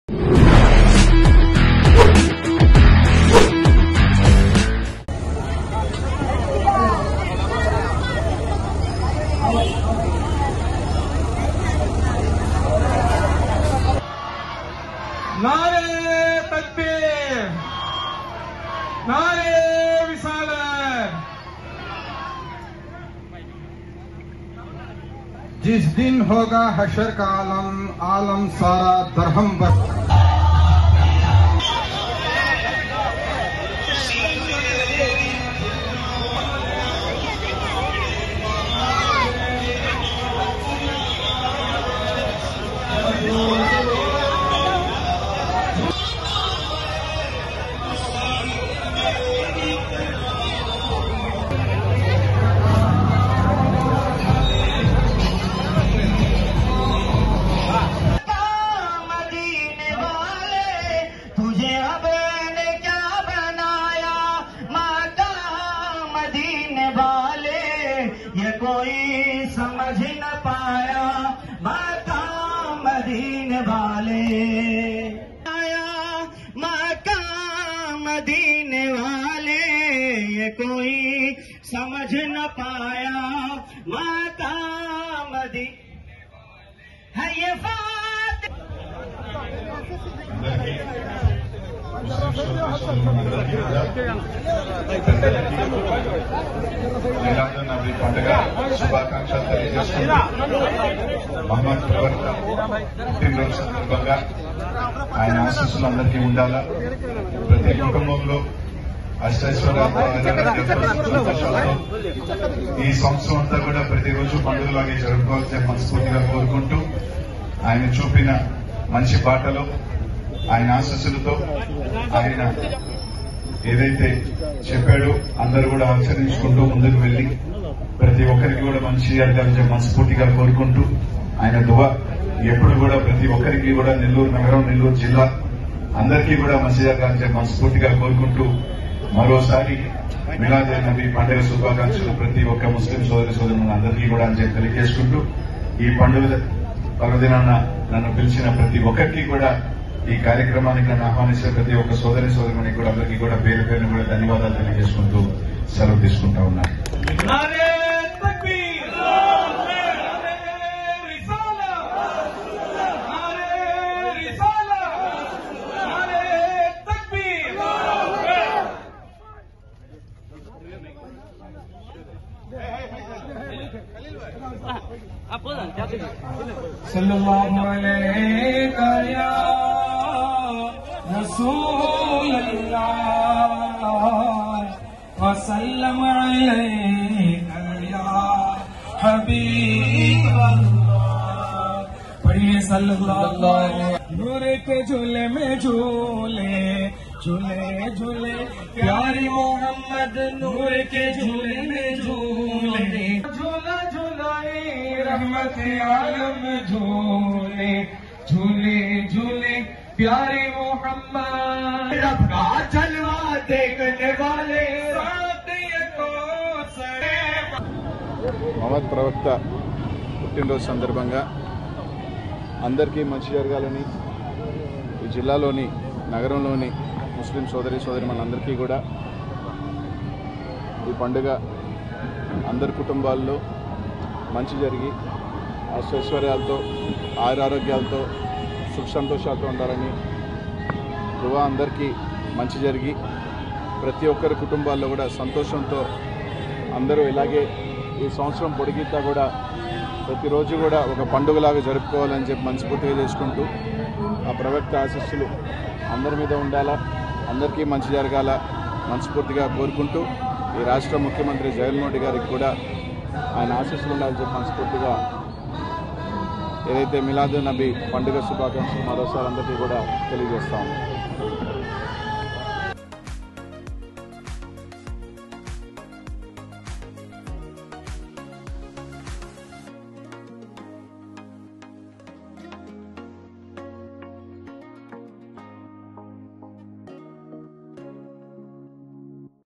మిలాద్ ఉన్ నబి వేడుకల్లో మంత్రి అనిల్ కుమార్ యాదవ్ | Minister Anil Kumar Yadav | RTV
RTV Nellore is a leading Trending Channel giving the latest news on Telugu Topics.